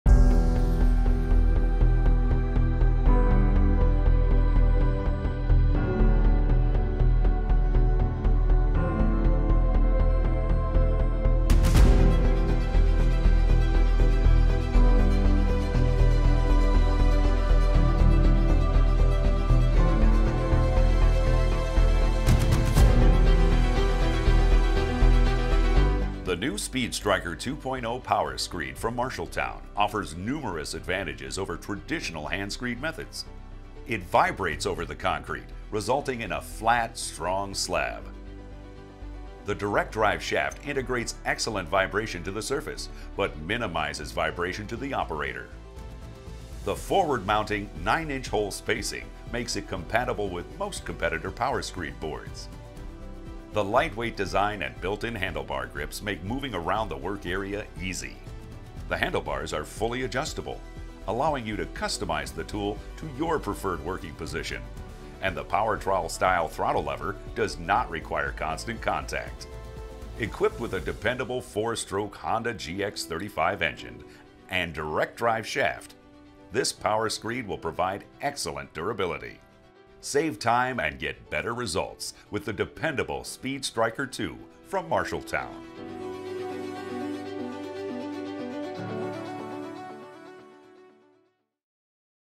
The New MARSHALLTOWN Speed Striker™ 2.0 Power Screed
The new Speed Striker 2.0™ Power Screed from MARSHALLTOWN offers numerous advantages over traditional hand-screed methods.
It vibrates over the concrete, resulting in a flat, strong slab¬.
The direct drive shaft integrates excellent vibration to the surface but minimizes vibration to the operator.
The forward mounting 9” hole spacing makes it compatible with most competitor power screed boards.
The lightweight design and built-in handlebar grips make moving around the work area easy.

The handlebars are fully adjustable, allowing you to customize the tool to your preferred working position, and the power trowel-style throttle lever does not require constant contact.

Equipped with a dependable 4-stroke Honda® GX35 engine and direct drive shaft, this power screed will provide extended durability.

Save time and get better results, with the dependable Speed Striker 2.0™ from MARSHALLTOWN!